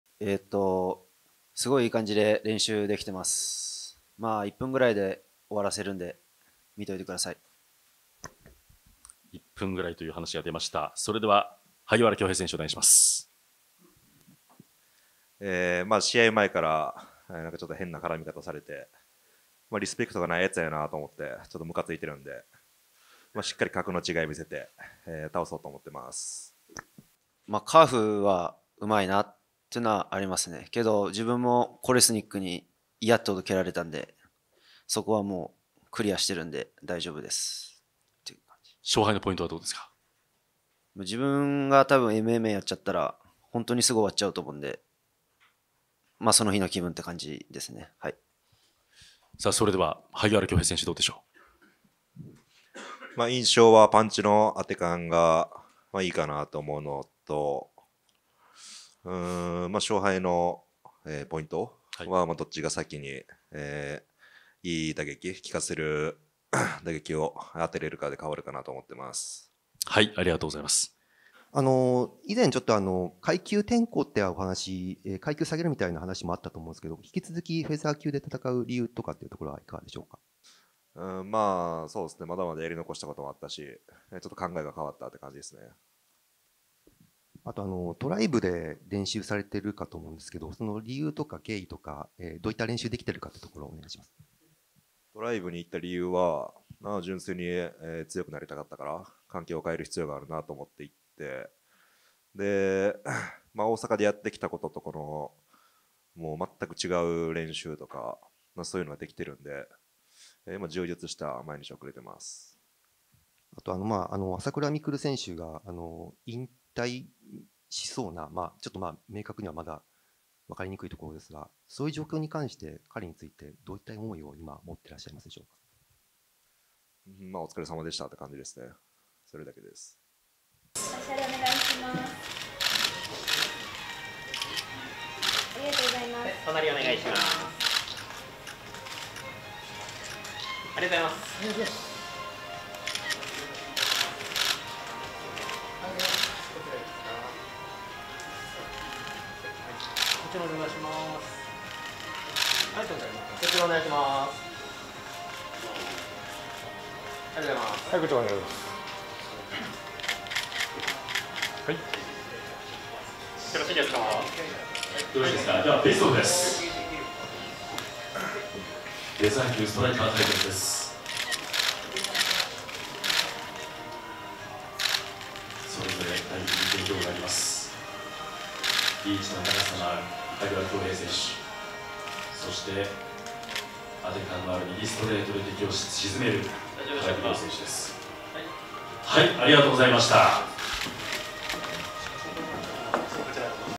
【RIZIN.48】萩原京平、高木凌の挑発にブチギレ！？「リスペクトがない奴やな」バチバチ煽り合いで一触即発 『RIZIN.48』追加対戦カード発表記者会見
9月29日開催の『Yogibo presents RIZIN.48』（さいたまスーパーアリーナ）の追加カード発表会見が21日に都内で行われ、フェザー級ワンマッチで対戦する萩原京平と高木凌が、マイクを通じて火花を散らせた。

このカードは先月28日の『超RIZIN.3』で発表され、この日の会見にも両選手がそろって登壇。先にマイクを持った高木が「いい感じで練習できています。1分くらいで終わらせるので見といてください」と挑発すると、萩原も「試合前から変な絡み方をされて、リスペクトのない奴やなと思ってムカついてるので。しっかりと格の違いを見せて倒そうと思ってます」とあしらった。

相手の印象を聞かれると、高木は「カーフキックは上手いと思うけど、自分も（ビクター・）コレスニックに嫌ってほど蹴られたので、そこはクリアしてる」と、昨年11月のコレスニック戦の反省を生かして、萩原対策に自信を見せる。勝敗のポイントについても「自分がMMAやっちゃったらすぐに終わっちゃうと思うので、その日の気分って感じですね」と余裕を漂わせた。

対する萩原は「印象はパンチの当て感がいいかな。勝敗のポイントはどっちが先にいい打撃を効かせられるか」と冷静に回答。また、3月に武田光司に判定で敗れて、階級を下げてバンタム級転向を示唆していたが、今回もフェザー級で戦う理由について「やりのこしたことがあったし、考えが変わった」と明かした。

この夏に練習拠点を大阪から東京に移し、多数の強豪選手が練習に集うジム「TRIBE TOKYO MMA」に移籍した萩原。「純粋に強くなりたかったから環境を変える必要があると思った。大阪と全く違う練習ができているので充実した毎日を送れています」と話した。

●9月29日『Yogibo presents RIZIN.48』対戦カード
（さいたまスーパーアリーナ）
【追加カード】
・バンタム級
元谷友貴vs.太田忍
・バンタム級
牛久絢太郎vs.佐藤将光
・バンタム級
金太郎vs.秋元強真
・フライ級
新井丈vs.エンカジムーロ・ズールー
・ライト級
矢地祐介vs.宇佐美正パトリック
・フェザー級
カルシャガ・ダウトベックvs.木下カラテ

【既報カード】
・ライト級タイトルマッチ
【王者】ホベルト・サトシ・ソウザ vs. 【挑戦者】ルイス・グスタボ
・バンタム級王座決定戦
井上直樹 vs. キム・スーチョル
・スーパーアトム級
伊澤星花 vs. 浅倉カンナ
・フェザー級
萩原京平 vs. 高木凌